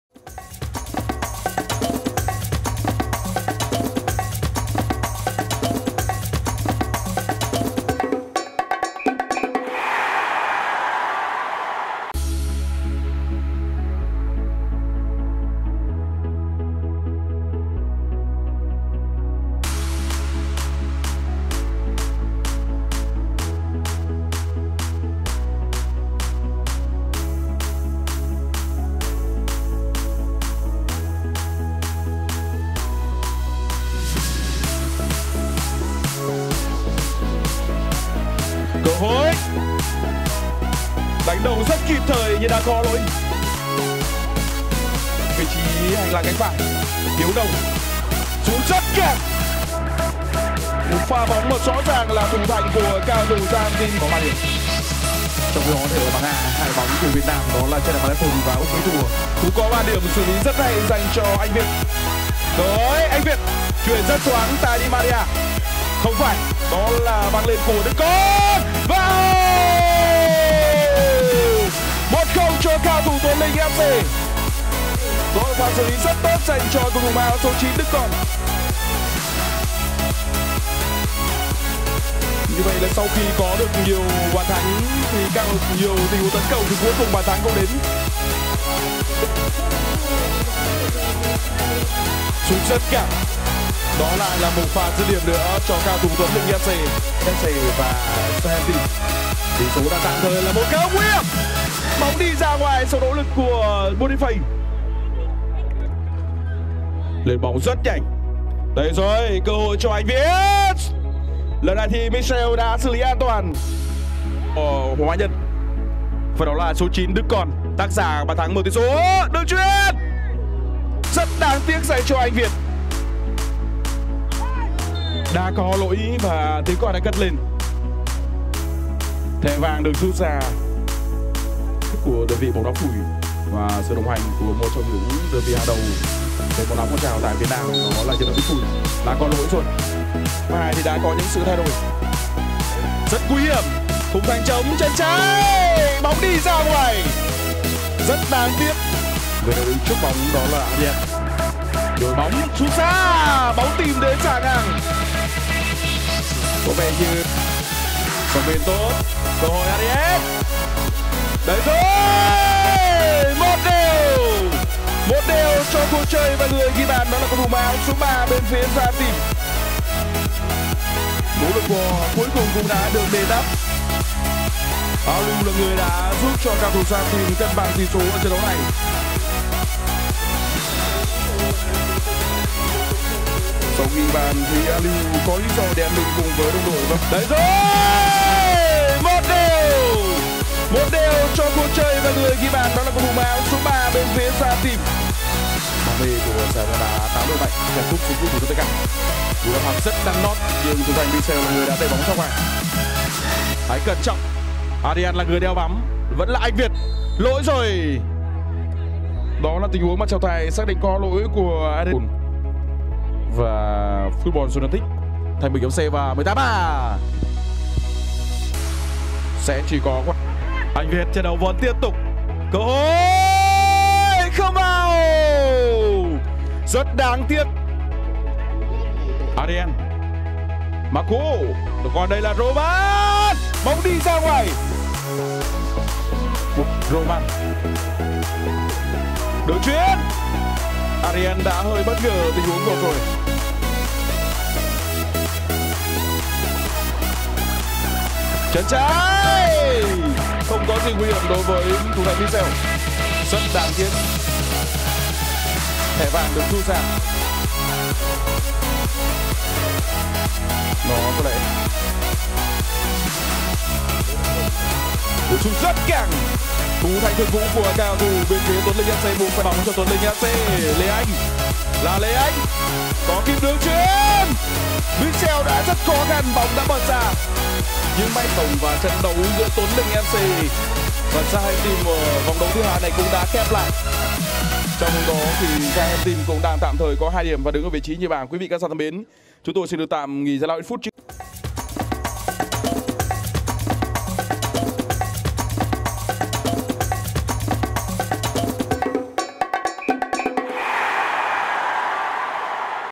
HIGHLIGHT Vòng 2 Bảng B | GIẢI BÓNG ĐÁ 8 ĐỘI MẠNH TRANH CUP SEEFOOD MỘT CHO TẤT CẢ.
🛑 TRỰC TIẾP | Vòng 2 Bảng B |
GIẢI BÓNG ĐÁ 8 ĐỘI MẠNH TRANH CUP SEEFOOD MỘT CHO TẤT CẢ.
➡ TUẤN LINH FC 🆚 GIAHTEM FC
⏱ 18:00 Ngày 30/05/2020
🏟 Sân Bóng | Football Arena| Q. Bình Thạnh
🎙 BLV : Anh Nhiên
🎬 Livebongda - Truyền Thông Thể Thao